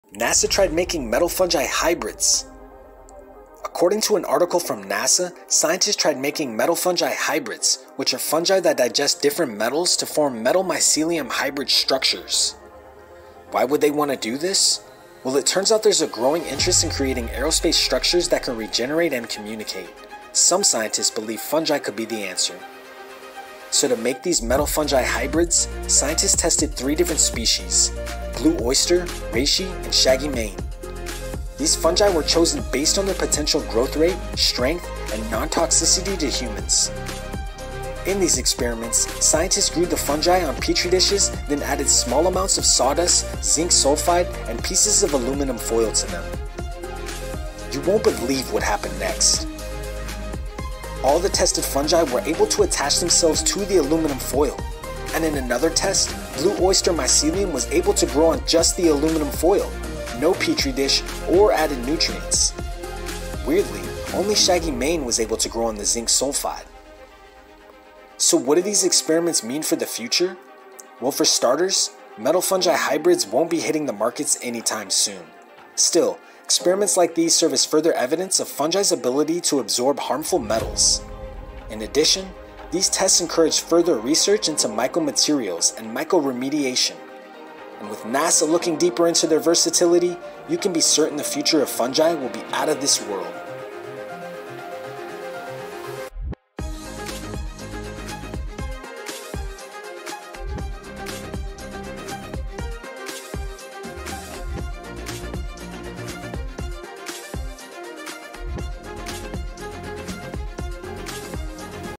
NASA Tried Making Metal Fungi Hybrids!!
A groundbreaking blend of science and nature that promises to revolutionize space exploration!
Don't miss out on the amazing results in this must-see video!